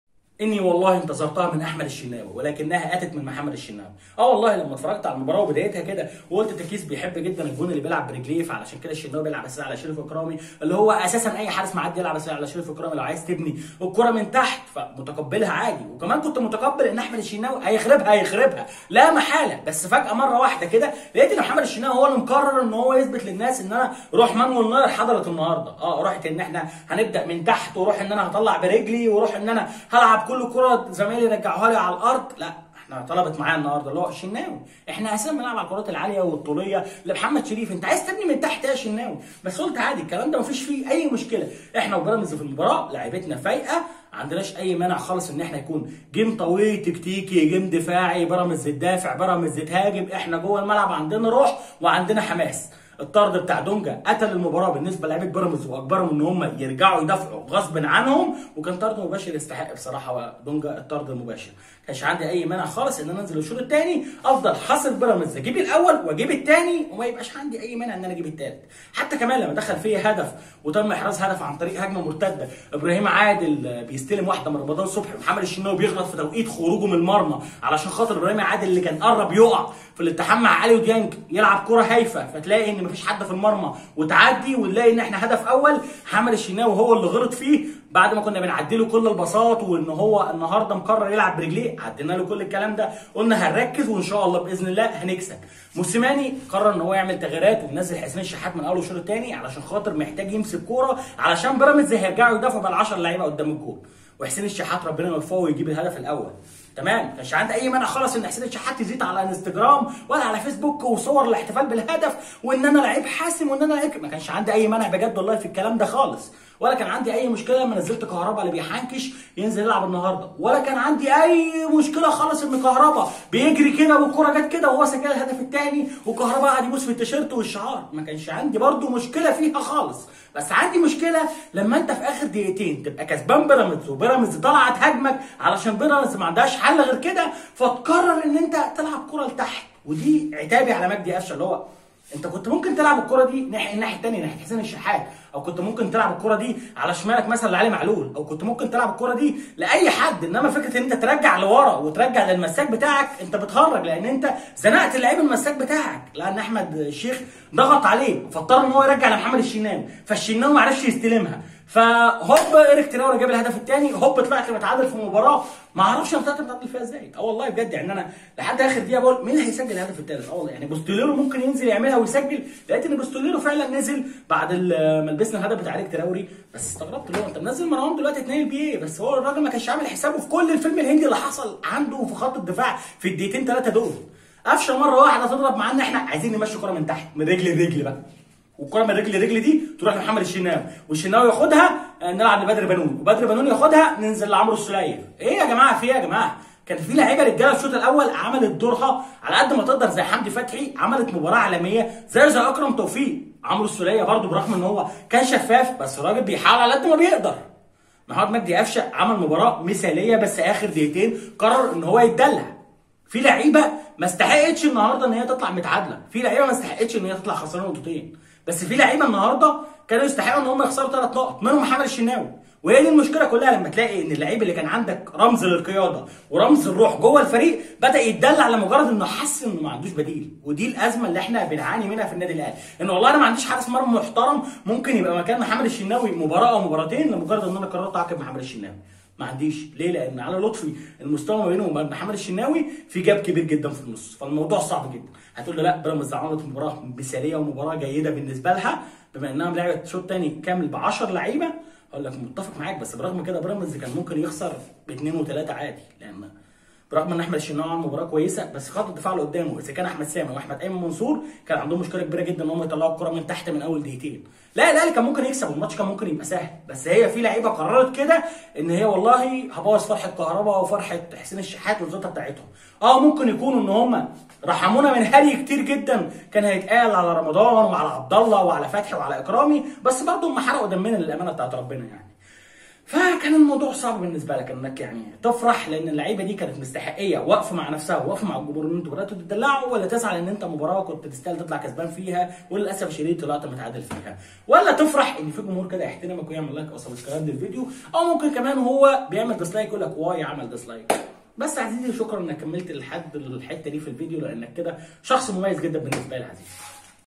رد فعل اهلاوي بعد تعادل الاهلي مع بيراميدز 2/2 كنت متقبل اي حاجه في الماتش ده الا دلع محمد الشناوي
تحليل الاهلي_بيراميدز ملخص مباراه اهداف مباراه الاهلي بيراميدز 2/2
الاهلي يعود إلى سباق الدوري_المصري لمطاردة المتصدر الزمالك و يفتتح العودة بتعادل غير منطقي امام بيراميدز عاند فيه الحظ النادي الاهلي